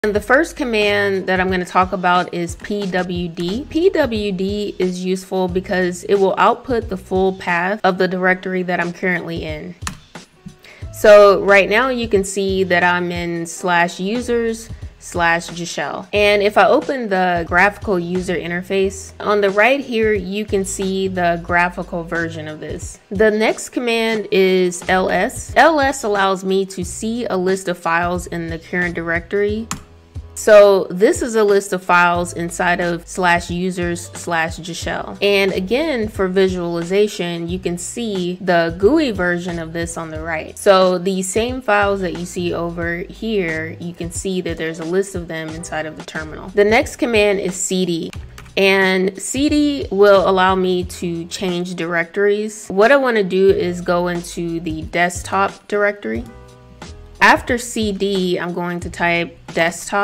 Useful terminal commands (clip)
💻  Using the command line terminal is a convenient way to navigate a computer’s file system along with working with tools like Git and GitHub. I list some common and useful ones in this video.